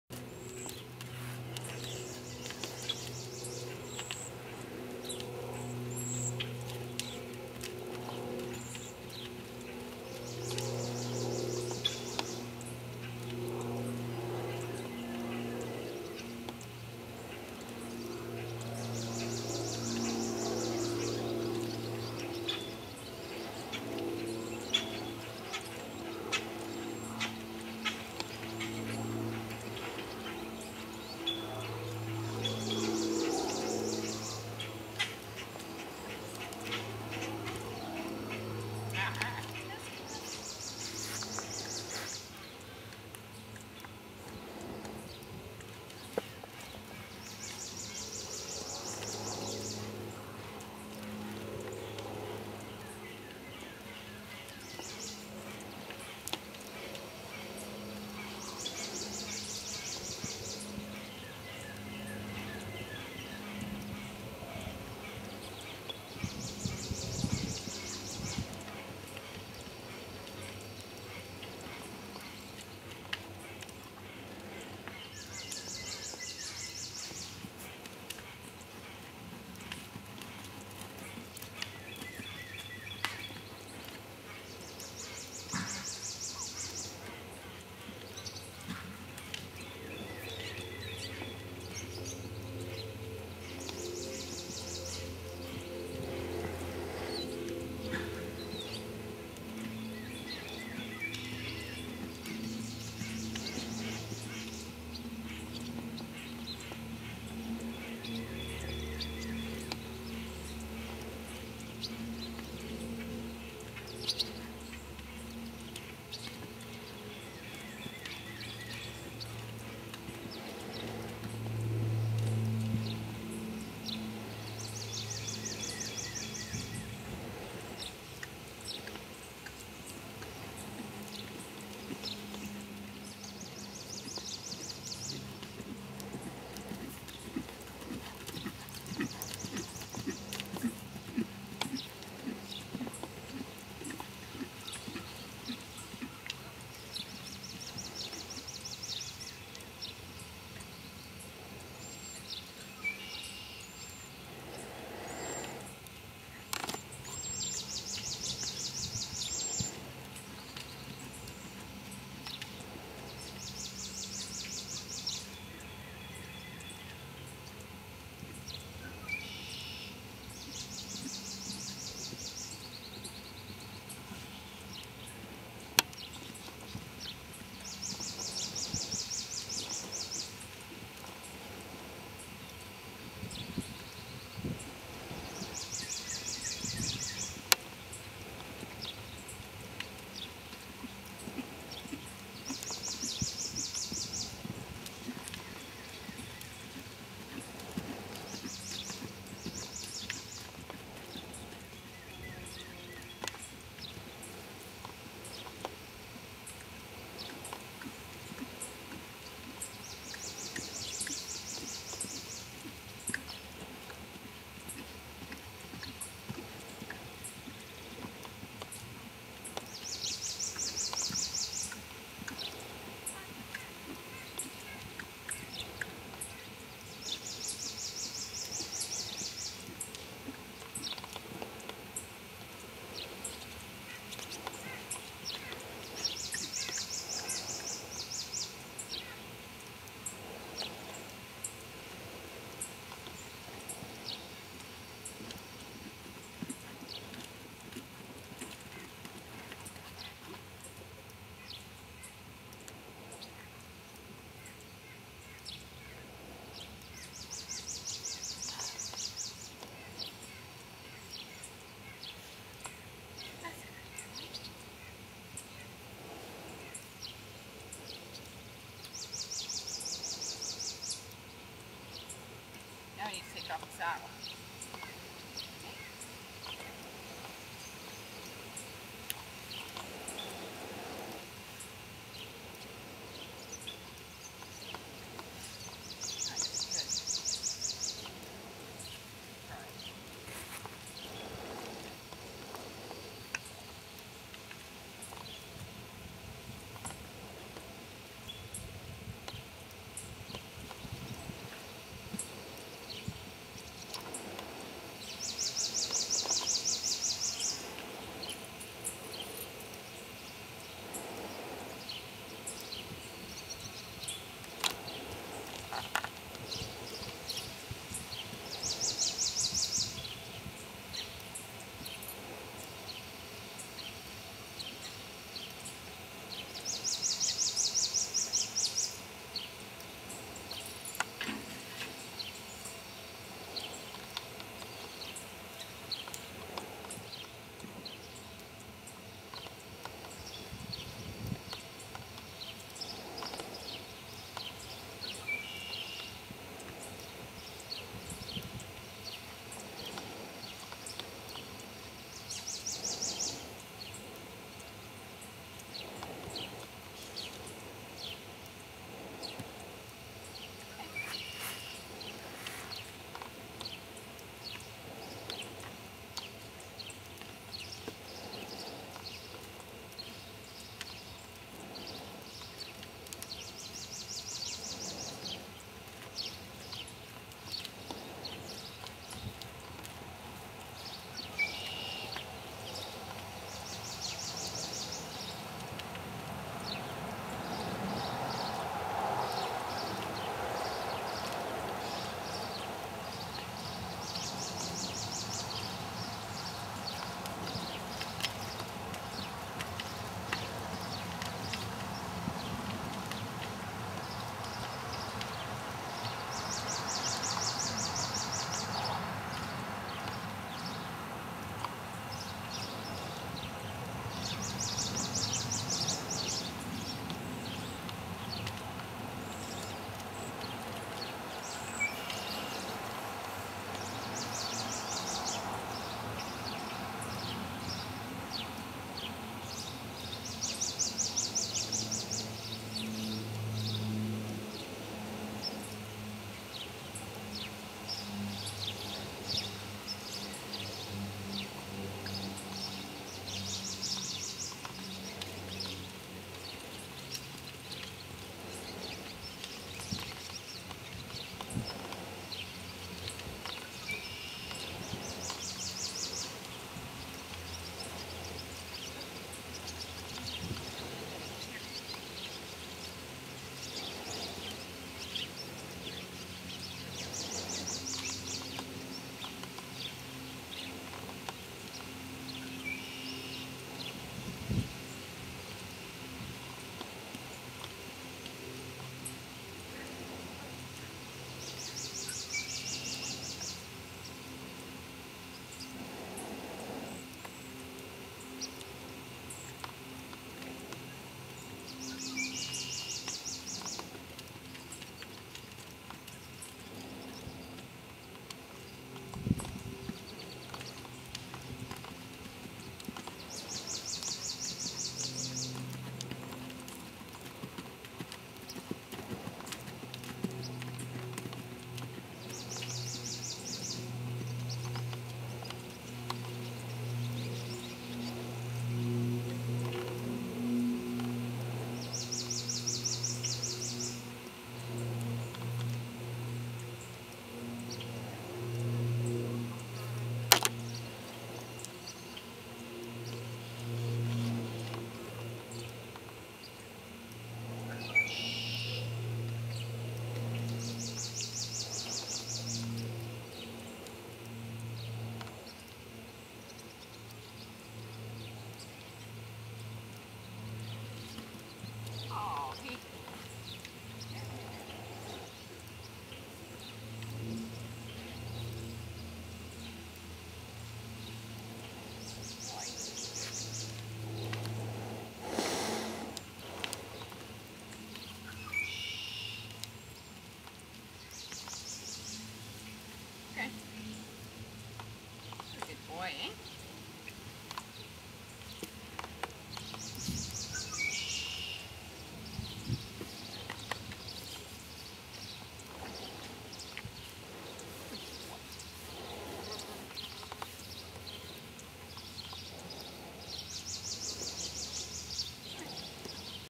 pico july 2009
Pico is a 7 year old half Arab. We practice Parelli natural horsemanship and I have my level 3 with my partner Pico. He is my very handsome boy and I love him.